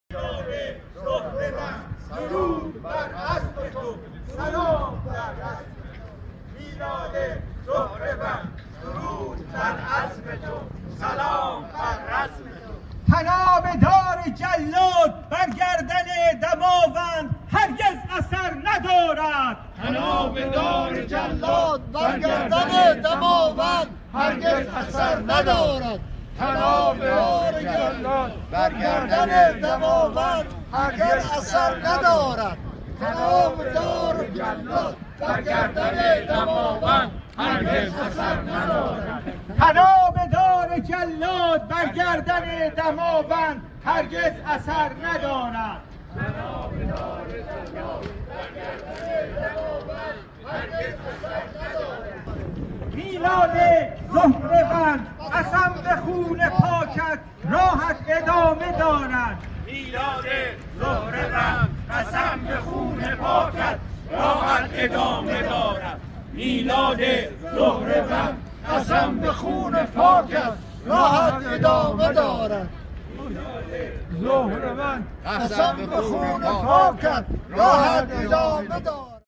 Stockholm—Nov 25, 2023:MEK Supporters Rally in Front of the Parliament in Support of Iran Revolution
Stockholm, Sweden—November 25, 2023: Freedom-Loving Iranians and MEK Supporters Held a Rally in Front of the Parliament in Solidarity With the Iranian Revolution, Condemning the Brutal Executions in Iran.